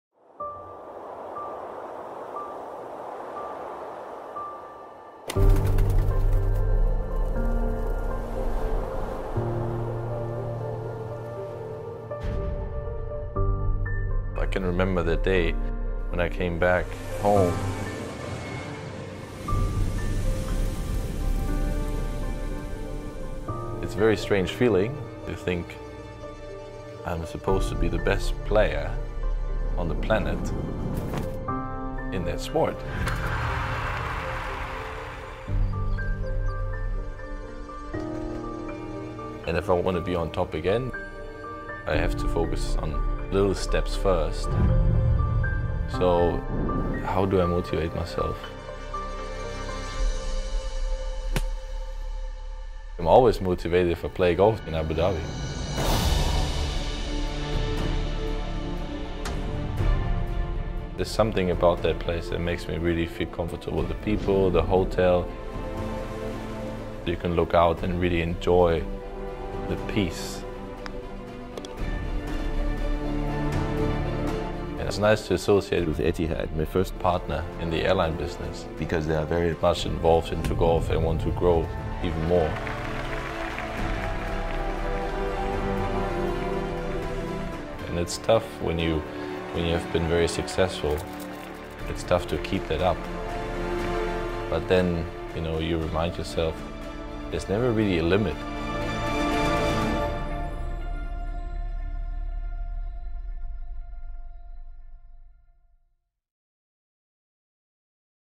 Martin Kaymer - Abu Dhabi Golf - Etihad Airways
Etihad Guest Ambassador, Martin Kaymer, flies to Abu Dhabi with us to explore the city's incredible golf courses.

Watch him share his story of success and his experience with Etihad Airways as he plays on the beautiful Saadiyat Beach Golf Club at the St. Regis.